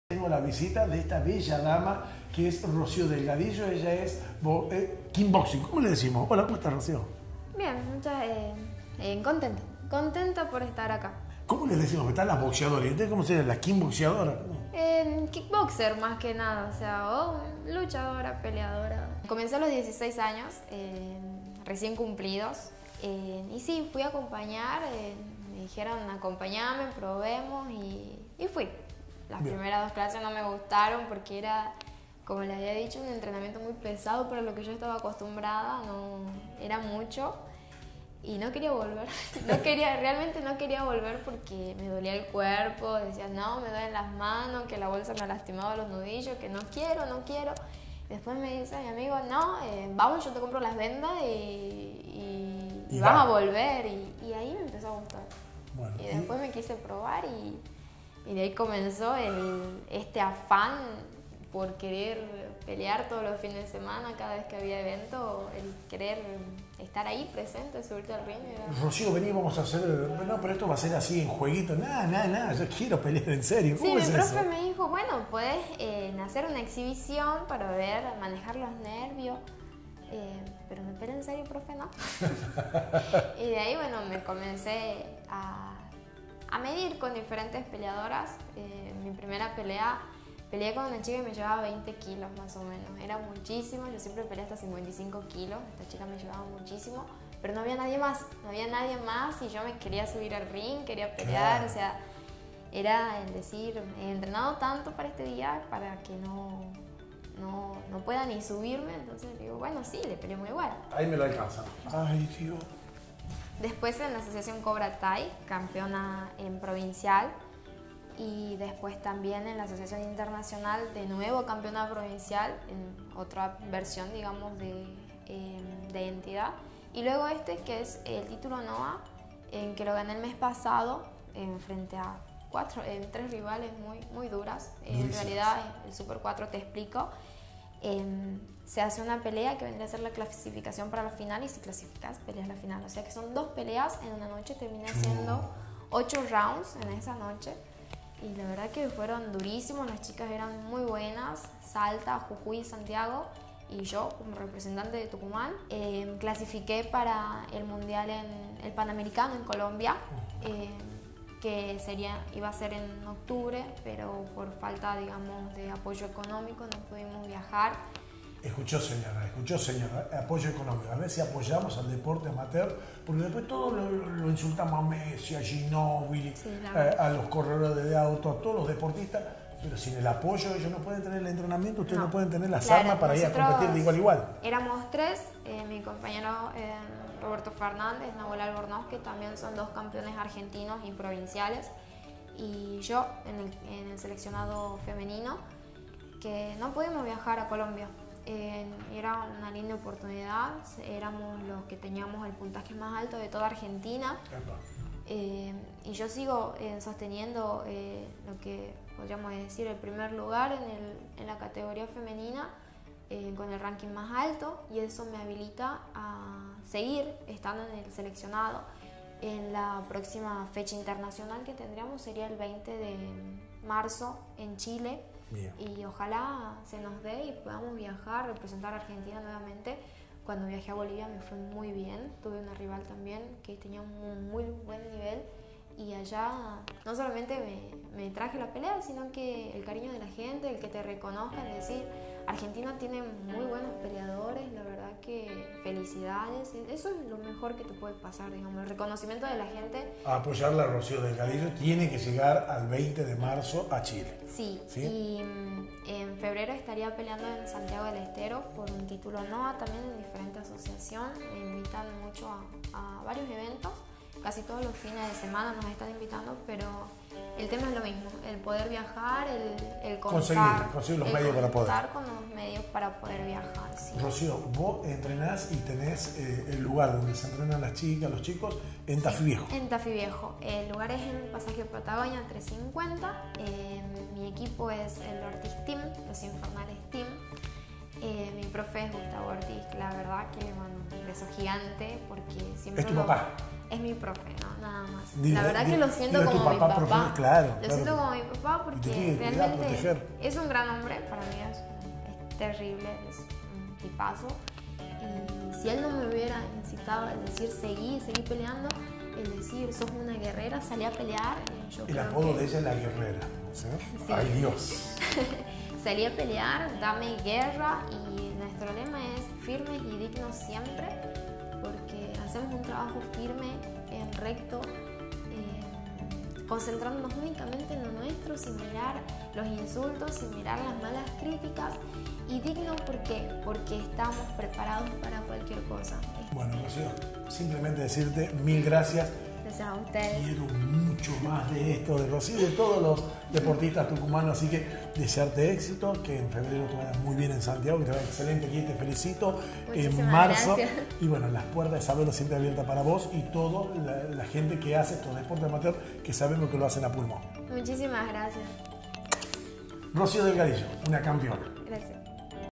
Rocio Delgadillo Campeona KickBoxing Sabelo
La flameante campeona del NOA tuvo un mano a mano con SABELO